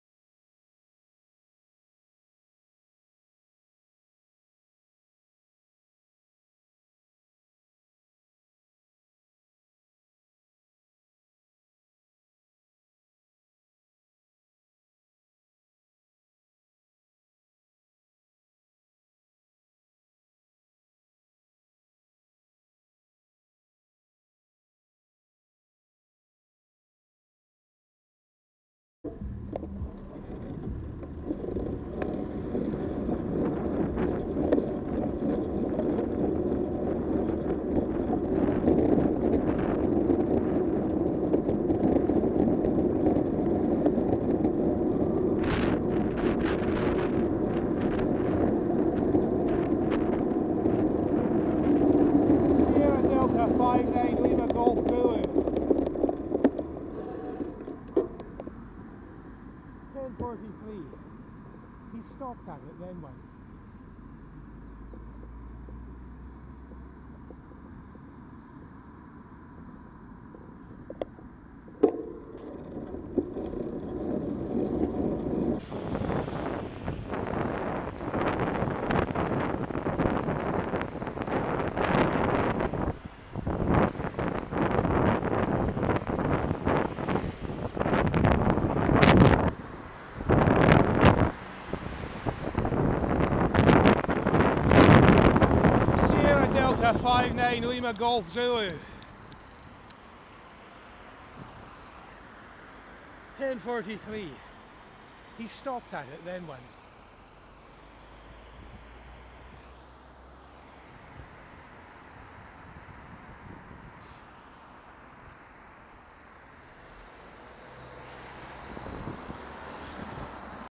SD59LGZ driver runs red light from stopped
Stops for the red light then changes their mind and drives through. I don't see this often but had two in an hour (SC16WJO).